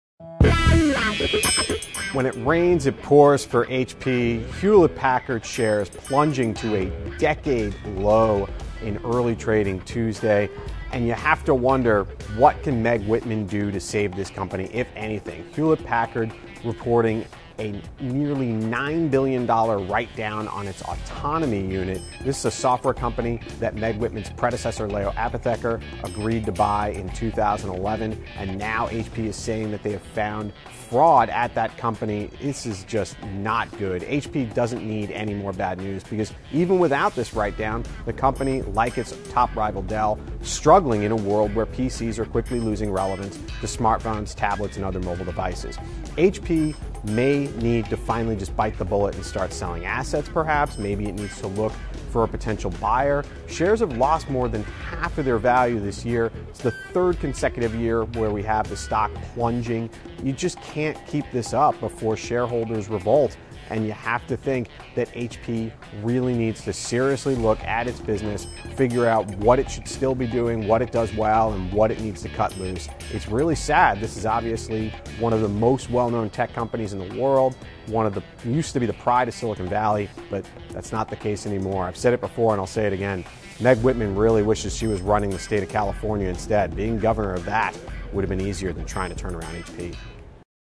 HP may have to split up
Once the pride of Silicon Valley, HP's write-down and sales woes could force the company to make drastic changes to stay competitive.